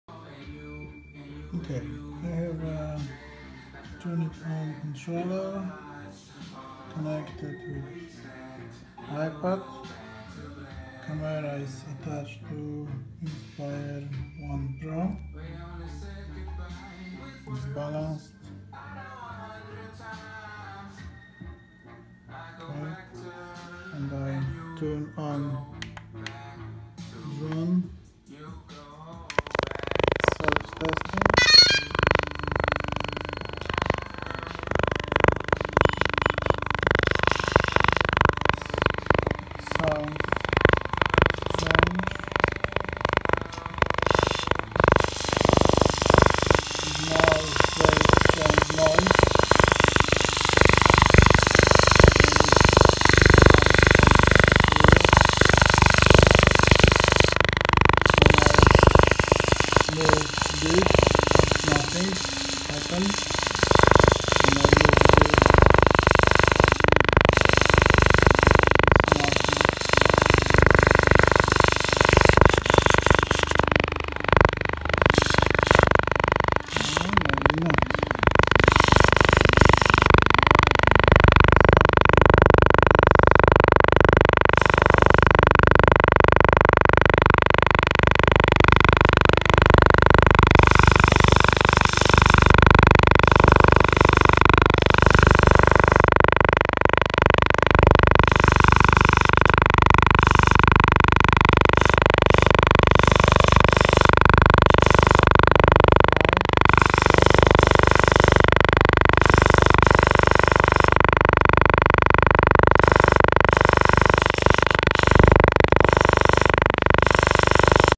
Drone zenmuse X5 camera failure
Drone camera failure, strange vibrations, geting hot